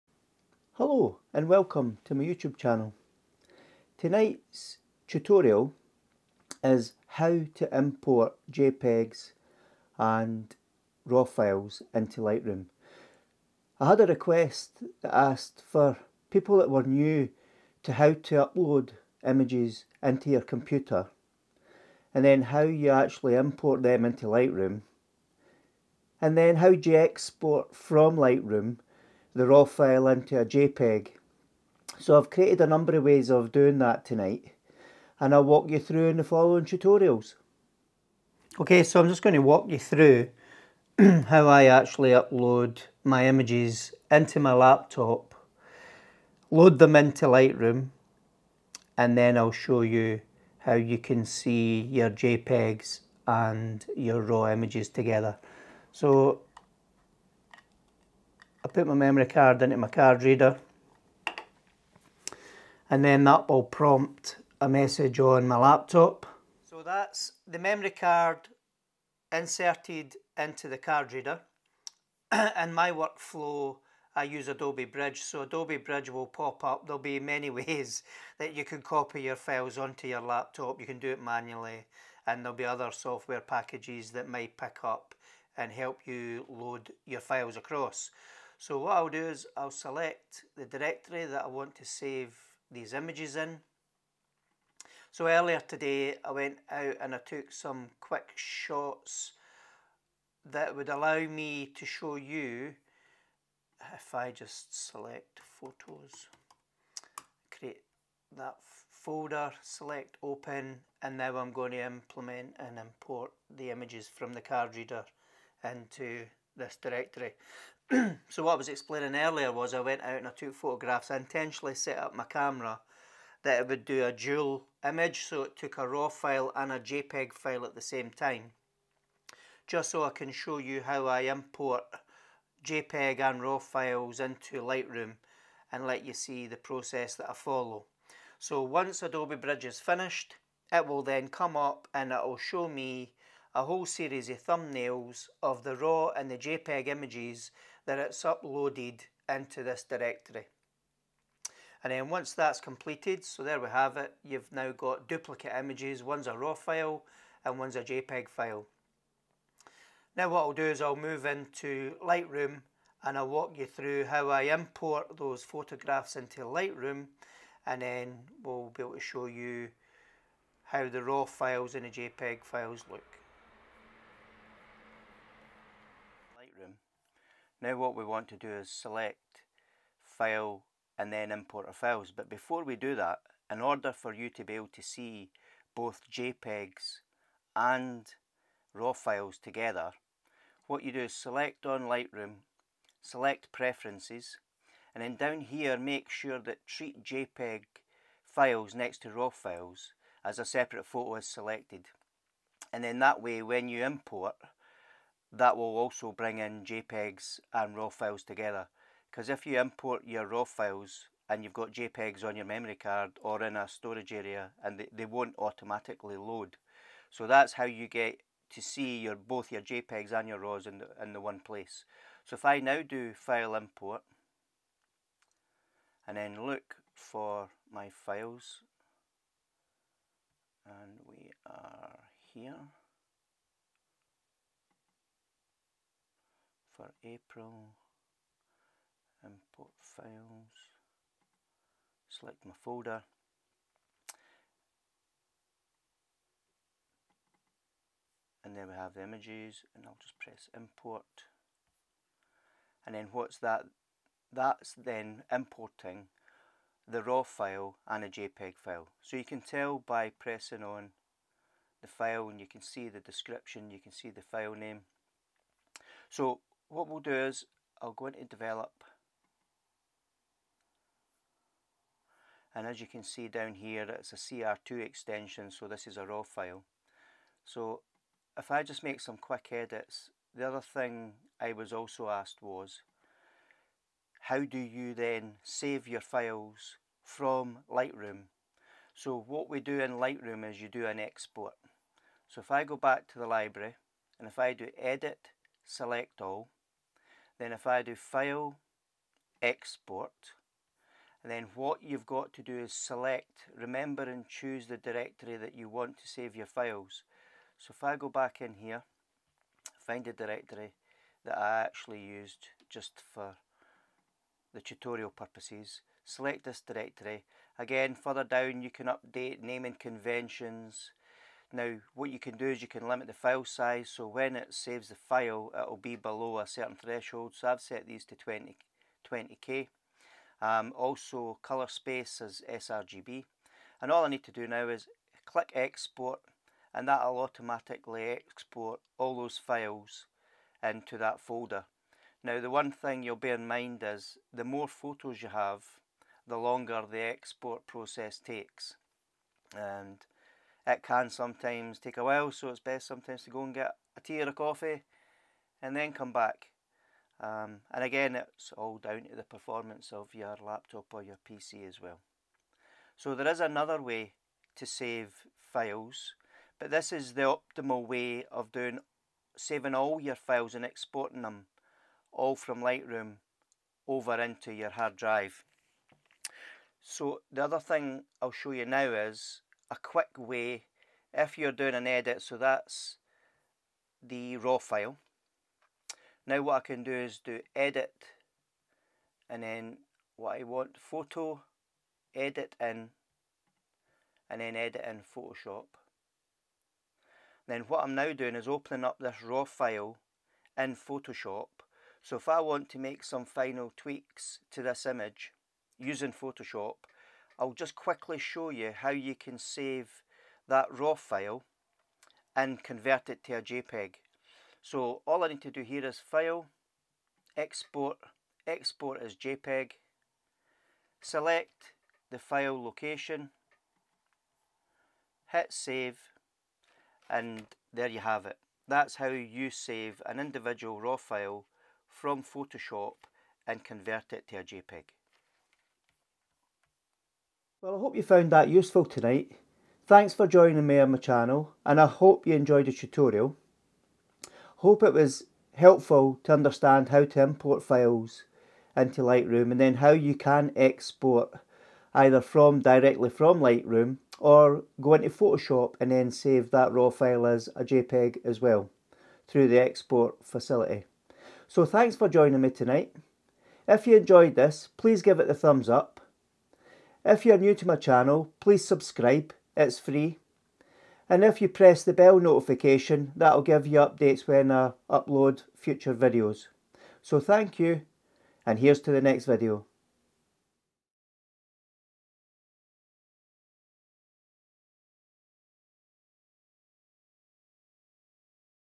Tutorial | How to Import Raw & Jpeg Files into Lightroom
In this tutorial I walk you through how to load images from a memory card into my laptop, then how to import those Raw and Jpeg images into Lightroom and help you follow a process to export and save your files from Lightroom and Photoshop.